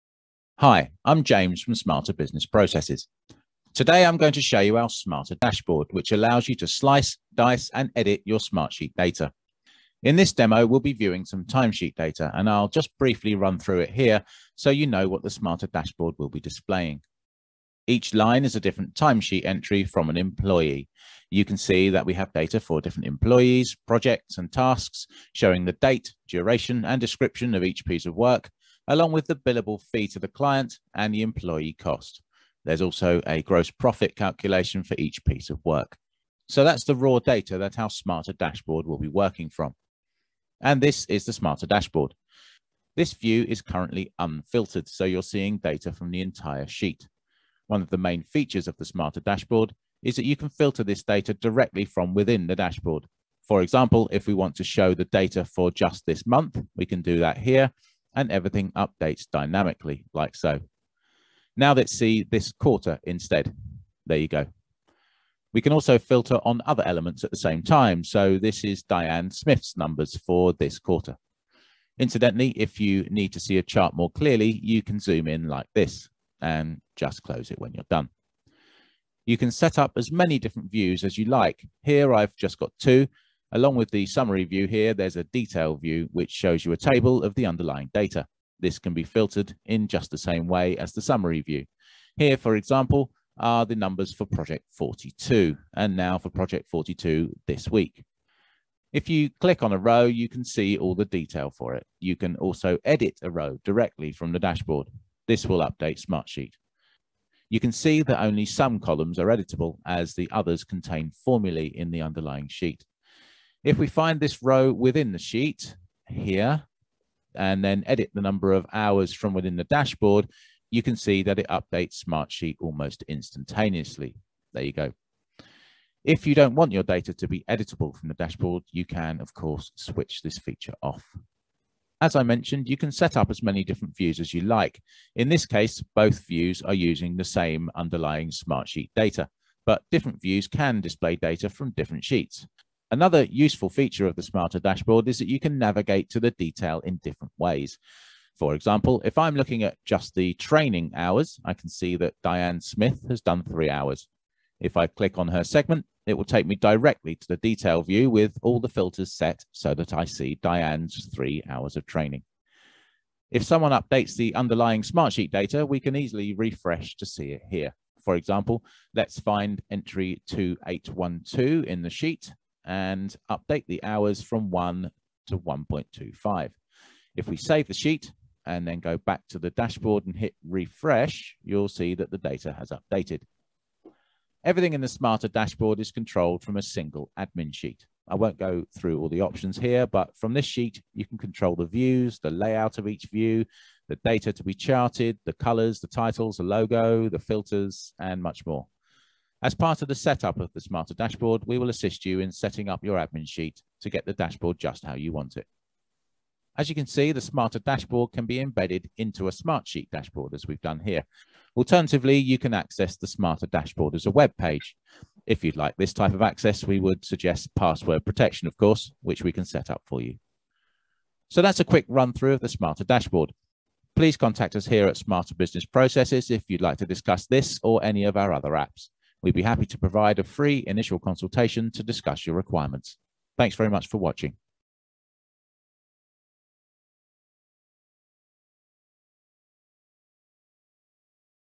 Smarter Dashboard
Our CTO Dr James Harris demonstrates how SBP have created an API solution that can slice and dice your data with visually clear and concise charts, graphs and tables.

With this tool you can dynamically filter data in a dashboard, and edit that data from within the dashboard itself. Smarter Dashboard makes it far easier for senior management to see what is happening and make crucial decisions based on real time criteria.

Something you want to see? Comment or leave a question below and we'll get straight back to you!